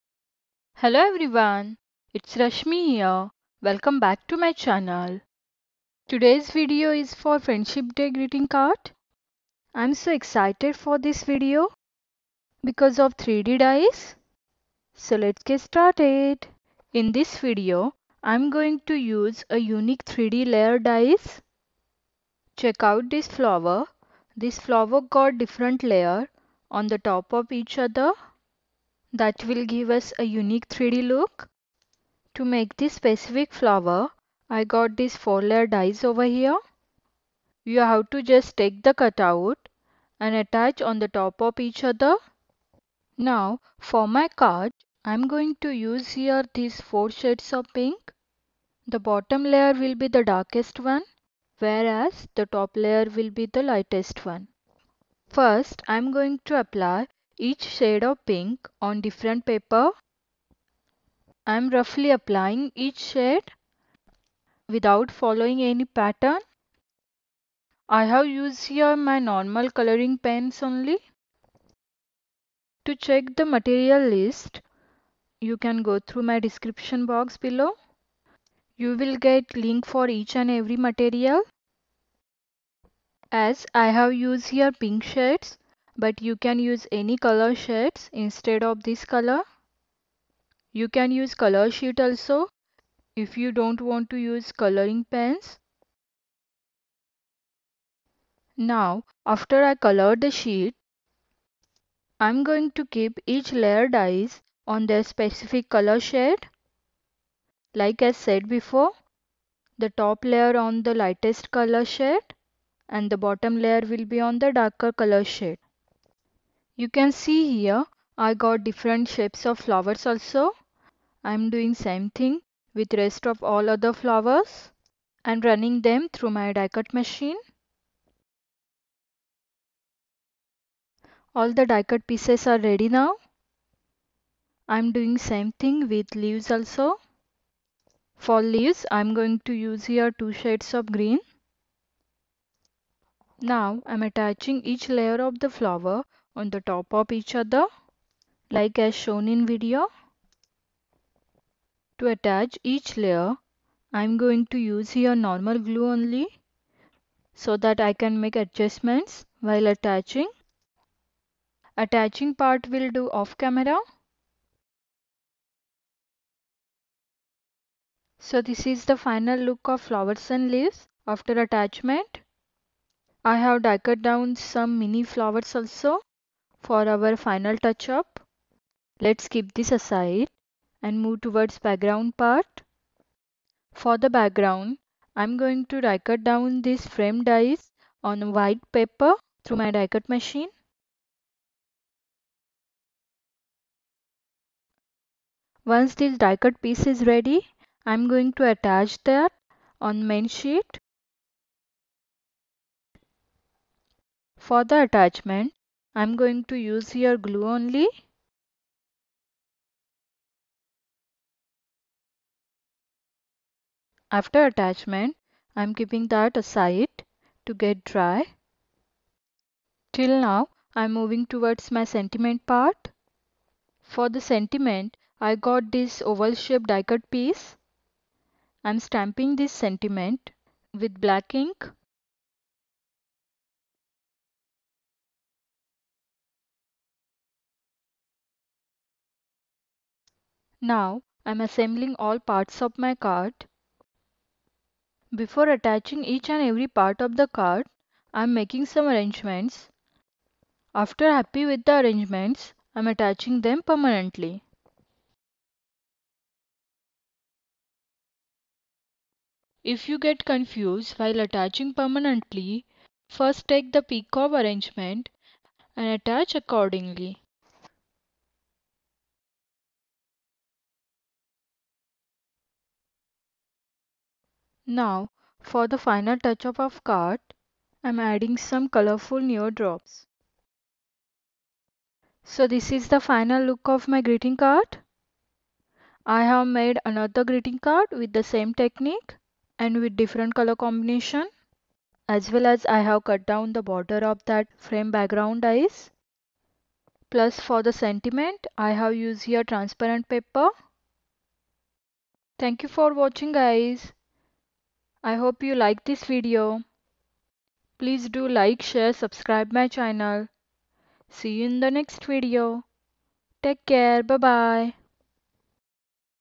DIY Greeting Card | Handmade | 3D flowers card
My online esty shop

Hello there, this is a greeting card for your lovely friends. With the same technique you can make any occasion card. In this video I have shown how to make your greeting card more beautiful by using 3d dies and layer dies.